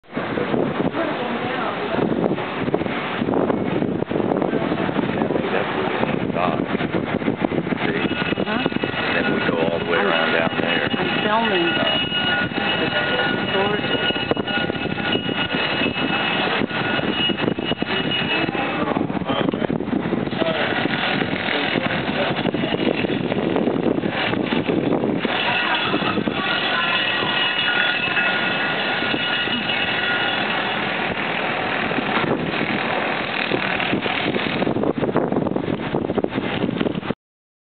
PART II Beautiful Port In Bermuda May 2010
It was absolutely breathtaking to come up on this gorgeous aqua marine colored water and such a beautiful day and wonderful weather and another cruise ship ahead of us facing us I think it was Norwiegen Cruise Lines.